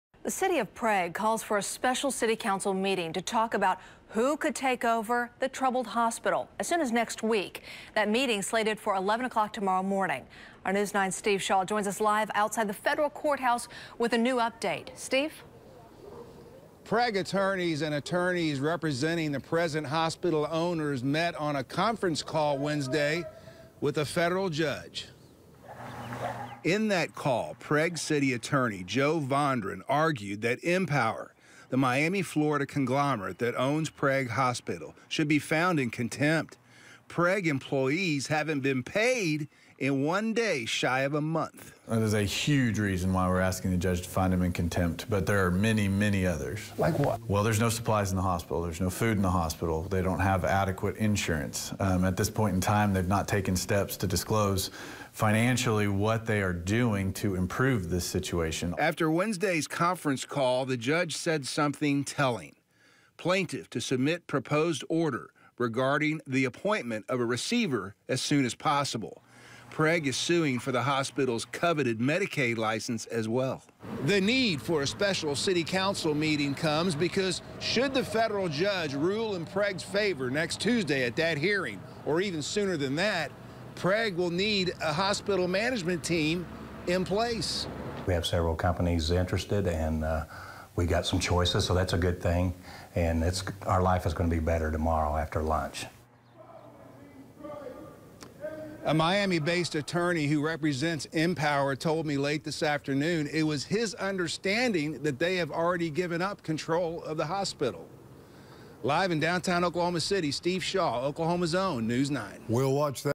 Prague To Hold Special City Council Meeting To Discuss Hospital Ownership
ᐸpᐳThe City of Prague has called a &ldquo;special&rdquo; city council meeting for 11 a.m. Friday March 1. It comes on the heels of a conference call involving a federal judge.&nbsp;ᐸ/pᐳ